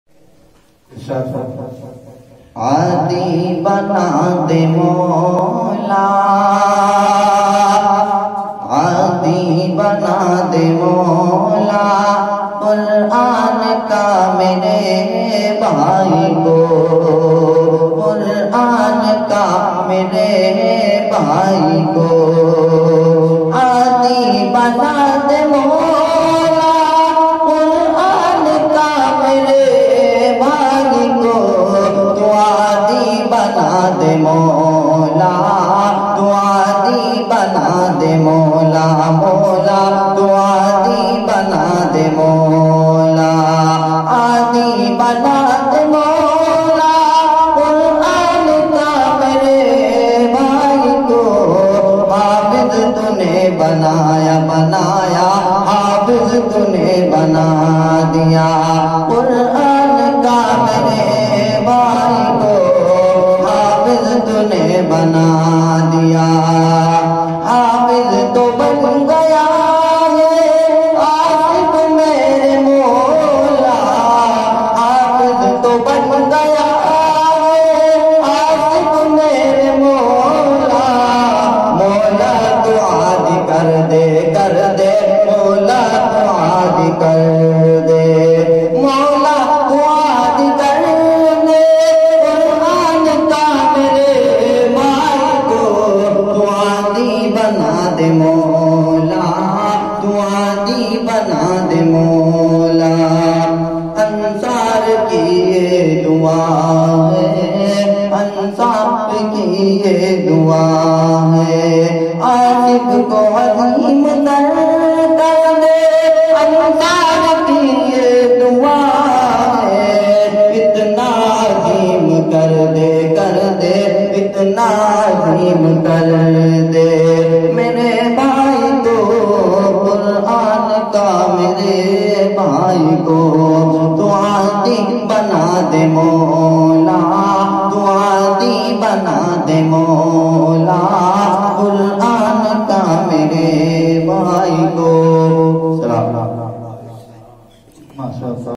10 फ़रवरी 2022
محمد آصف کی تکمیل کے موقع پر  انصار کیا خوب اشعار پڑھے ماشا ٕاللہ